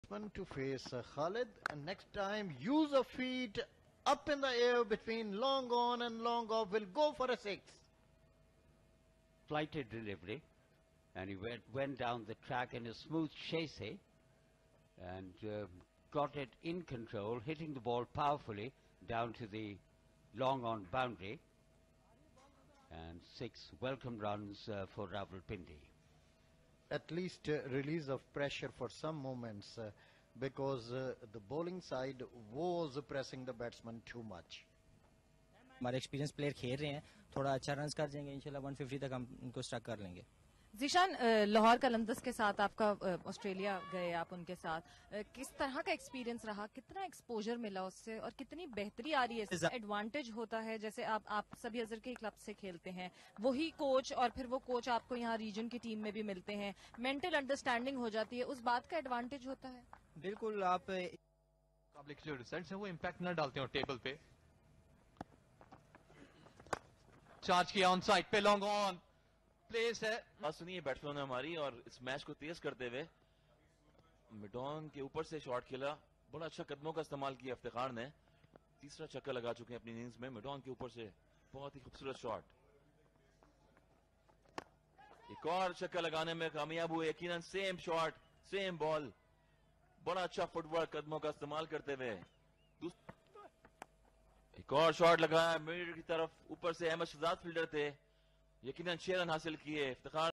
iftikhar ahmed 6 sixes in 6 Ball National T.20 Cup 2017
Iftikhr Ahmed smashes 60 with 6 sixes in National Twenty Cup for Rawalpindi against Lahore Blues.